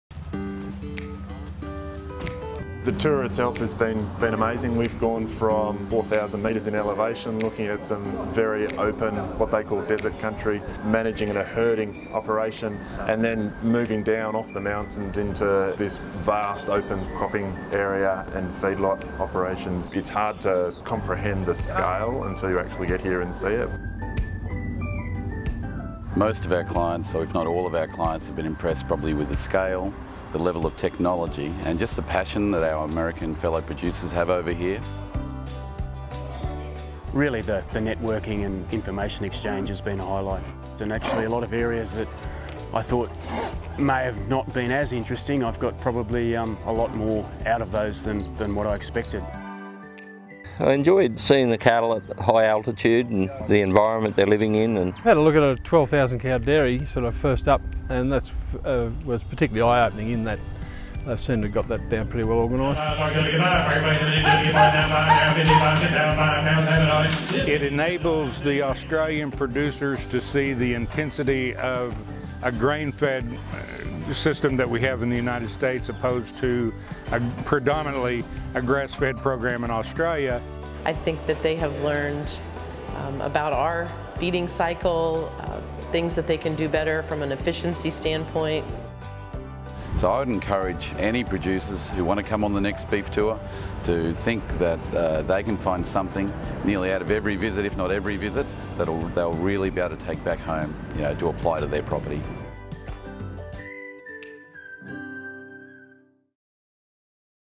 Rabobank US Beef Tour
In May 2019, a group of Australian beef producers and Rabobank staff including RaboResearch analysts Don Close and Angus Gidley-Baird, travelled through the US to learn about developments in technology in the beef sector that are driving production and profitability.